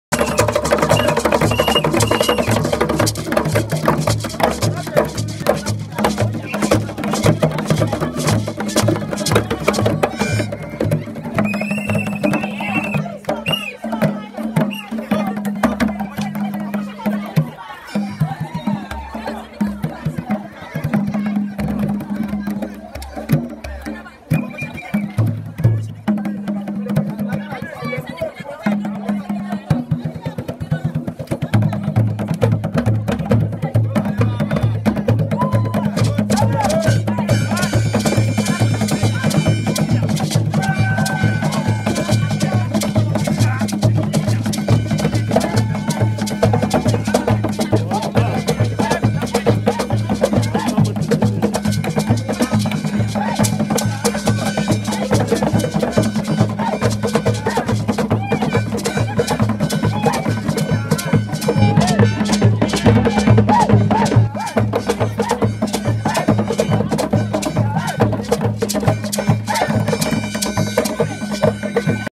Kabaka’s Birthday Run 2022: Jolly Mood In Lubiri Mengo
People enjoying the day at Lubiri Mengo during the Kabaka Birthday Run 2022